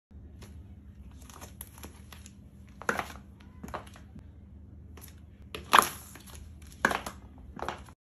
Experiment High Heels vs Ice Cube /Crushing Crunchy & Soft Things by Shoes
Warning ! Dangerous Crash Test، Don’t try this by yourself ! Experiment High Heels vs Fish toys / Crushing Crunchy & Soft Things by shoes !